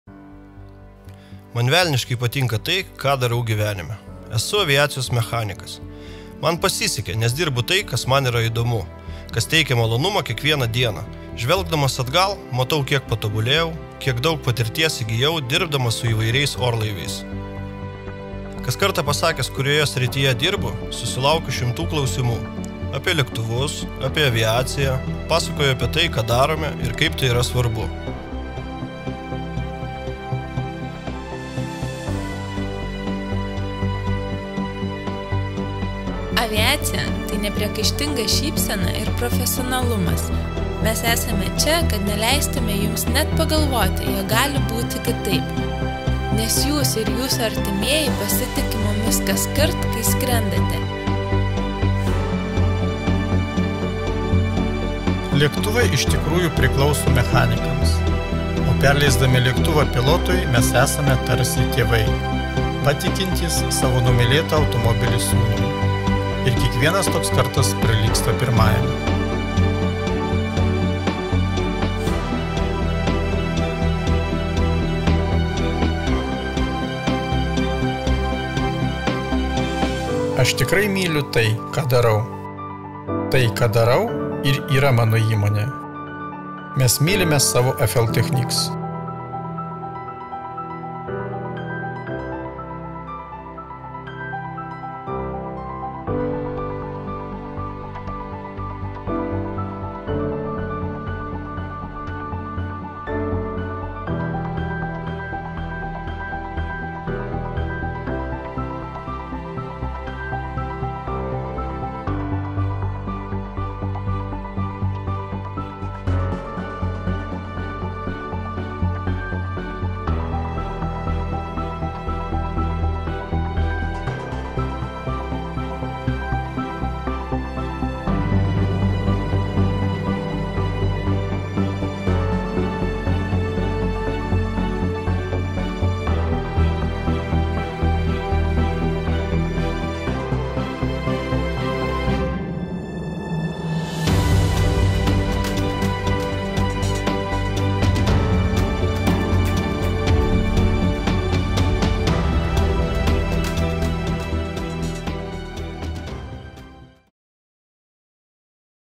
Mes mylim FL Technics!
Aviacija – tai ne tik šiuolaikinės technologijos ir skrydžiai. Pirmiausiai – tai žmonės, emocijos ir tradicijos.

Nufilmuota FL Technics kolektyvo pastangomis specialiai "Aš myliu savo įmonę" konkursui.